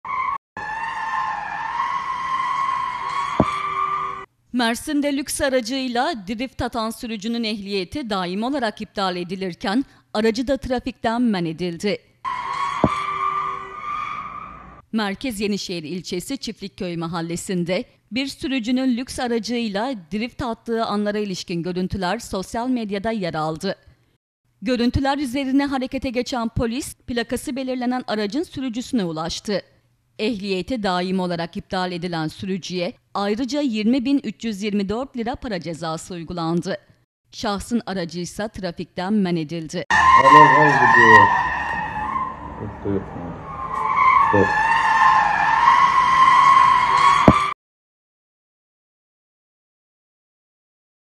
LÜKS ARAÇ İLE DRİFT ATMAK PAHALIYA MALOLDU
Mersin'de lüks aracıyla drift atan sürücünün ehliyeti daimi olarak iptal edilirken, aracı da trafikten men edildi.